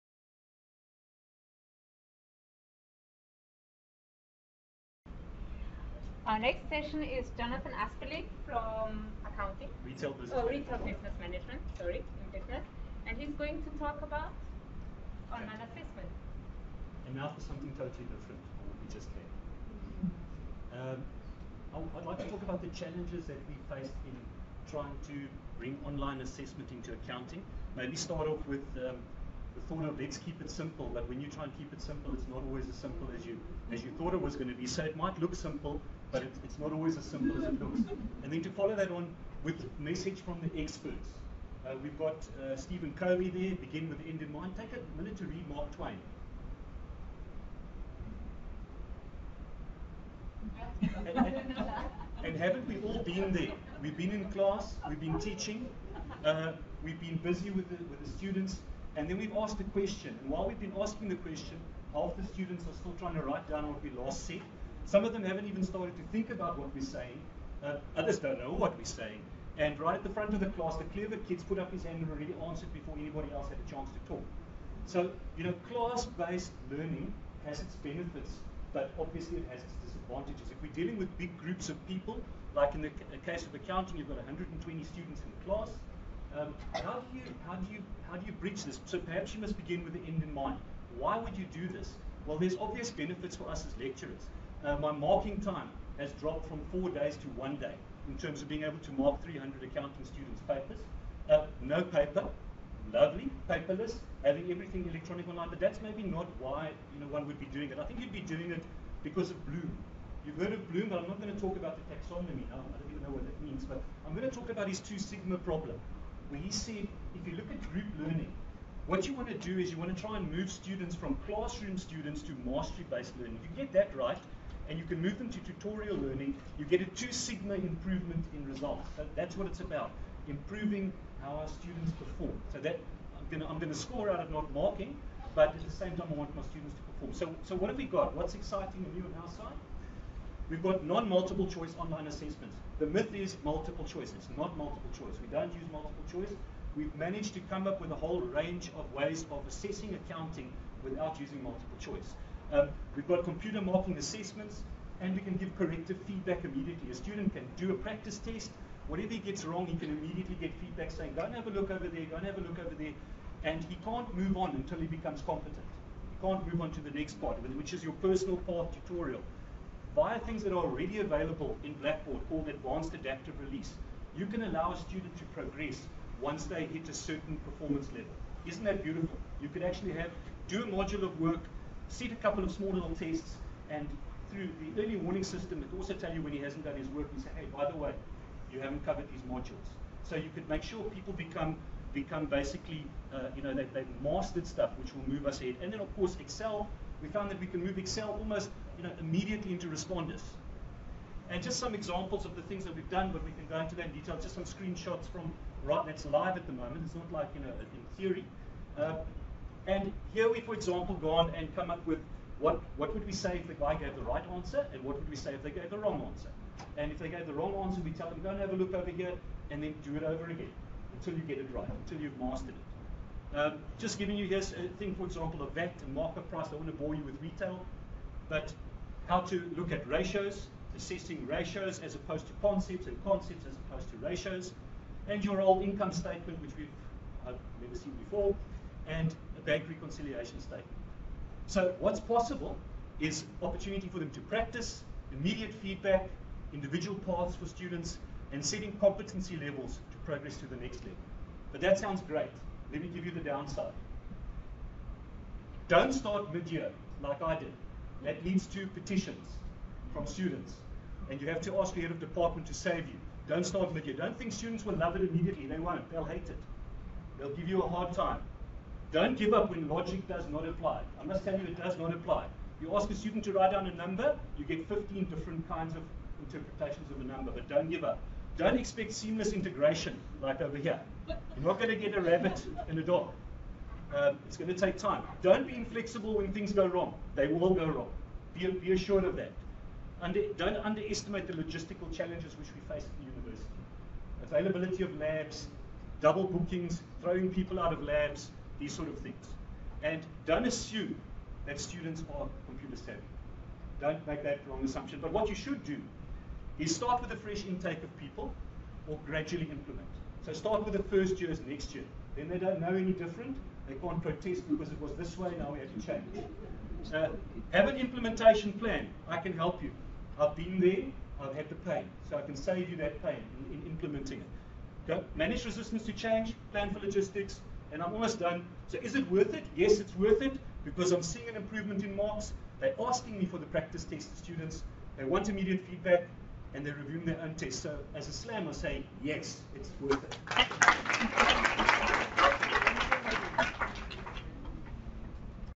Online assessment works by Jonathan Aspeling
Technology slam session at the 3rd Teaching and Learning with Technology Day at CPUT on the 6th of November 2013.